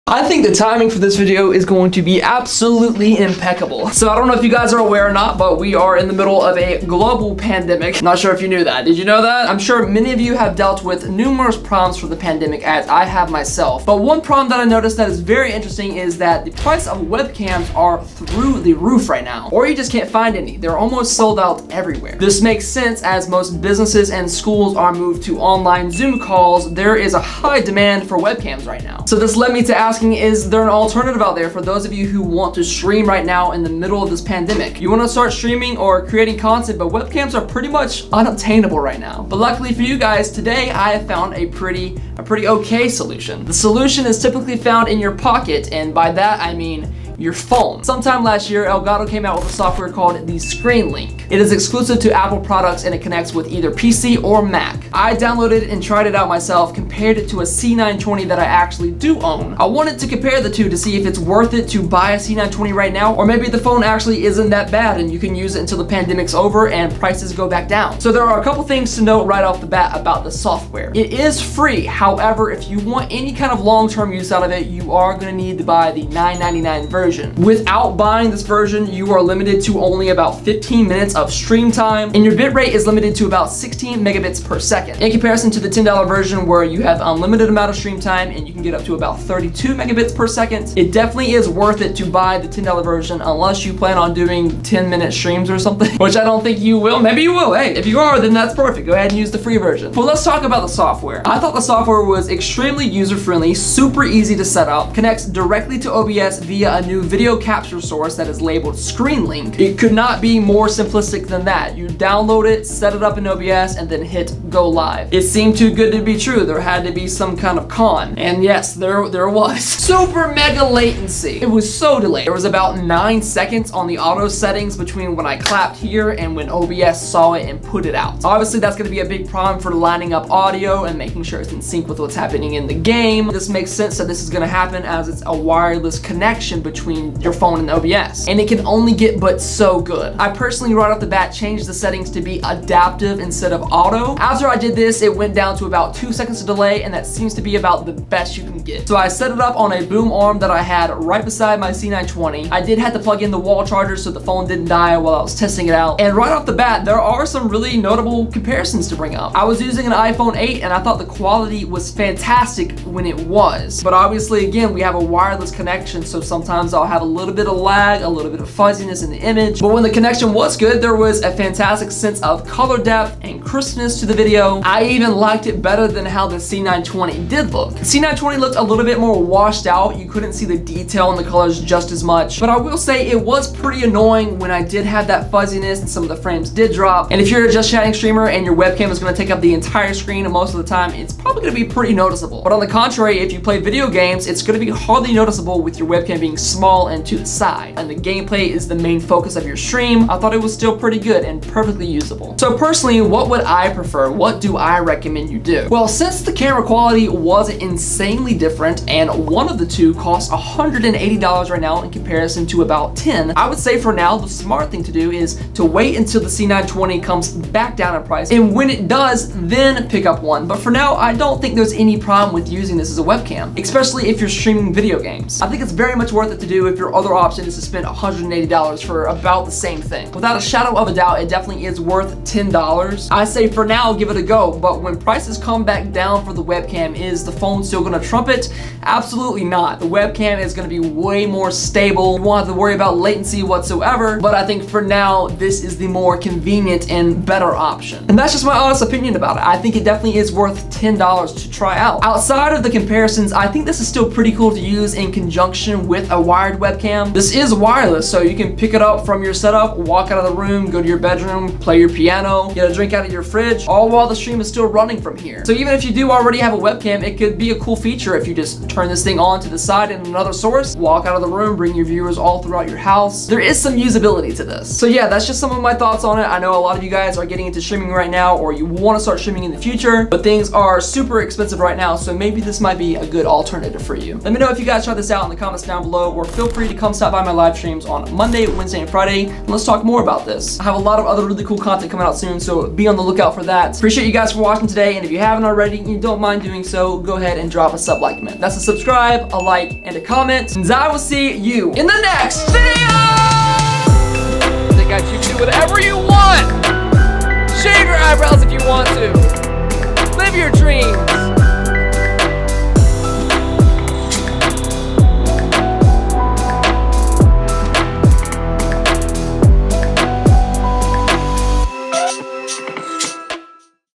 Webcams Too Expensive? Use Your IPhone!
Today. we take a look at the Elgato Screen link! This app allows you to stream DIRECTLY from your phone to OBS! Super easy to set up, and is MUCH cheaper than a webcam right now, but is it ACTUALLY worth it?